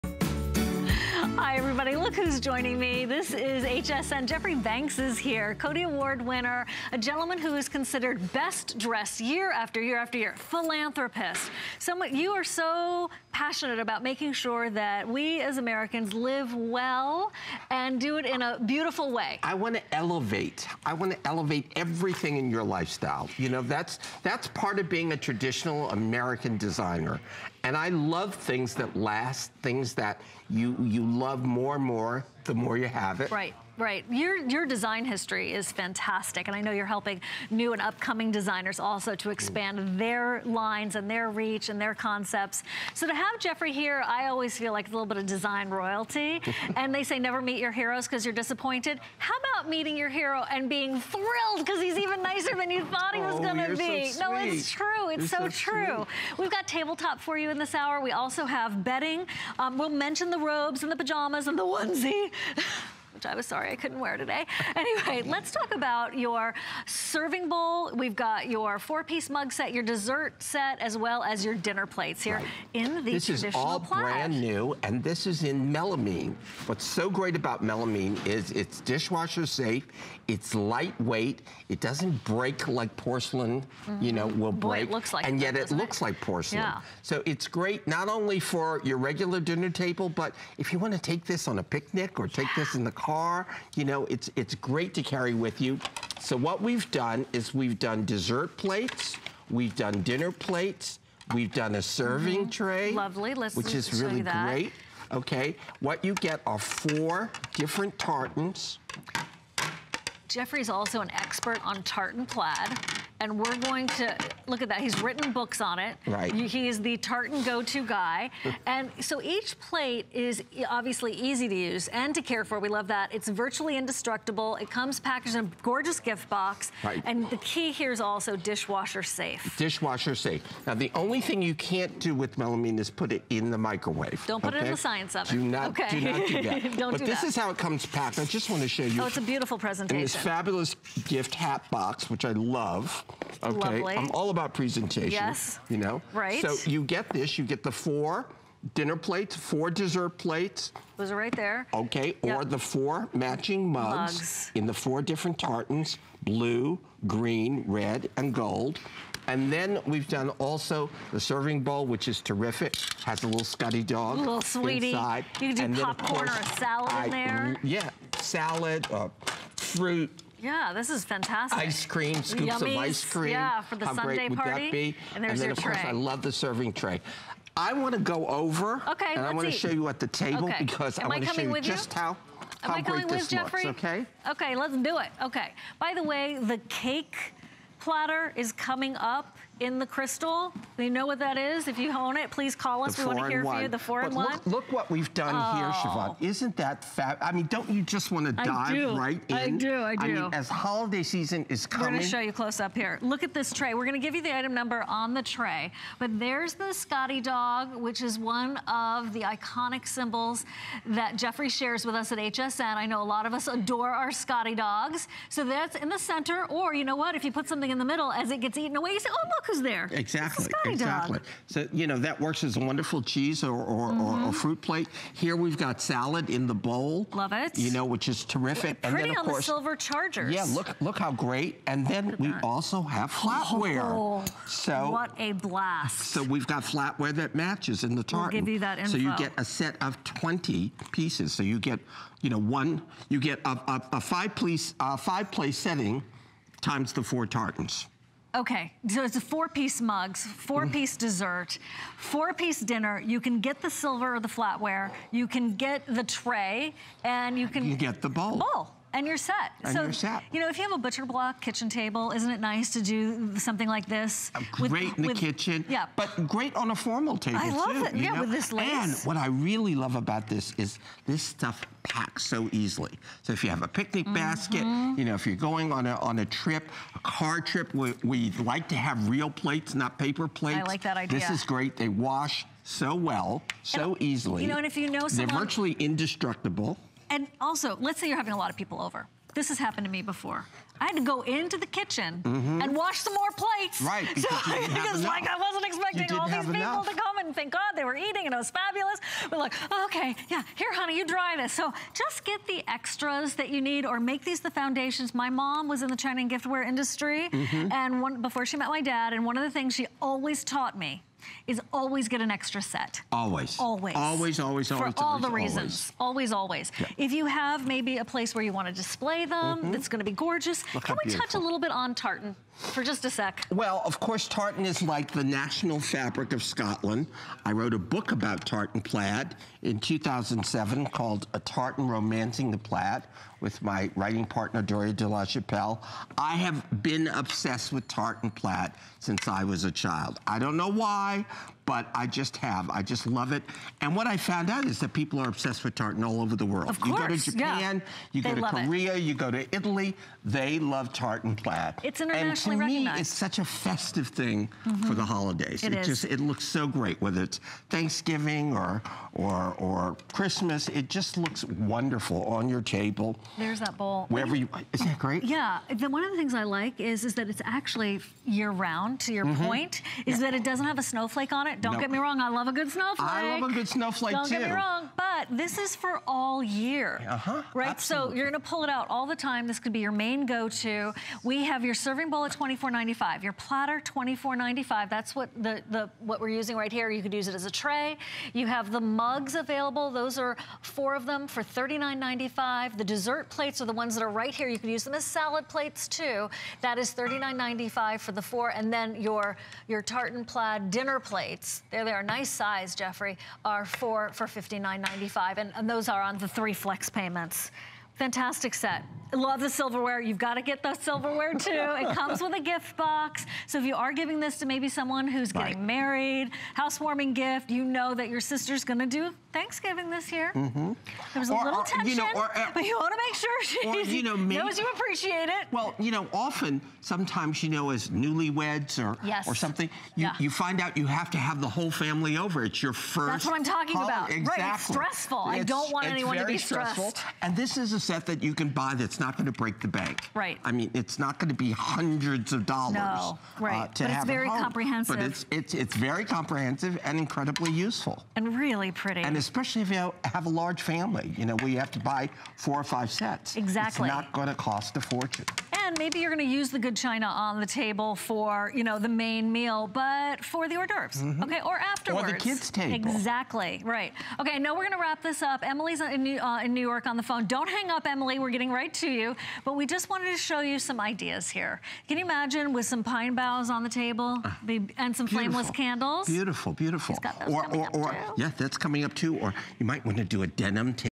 Jeffrey Banks Designer Plaid 4piece Dinner Plates
Jeffrey Banks Designer Plaid 4piece Melamine Dinner Plates
Timeless style is such a hallmark of class. Add your own fun take on tradition with these plaid dinner plates. They're made from...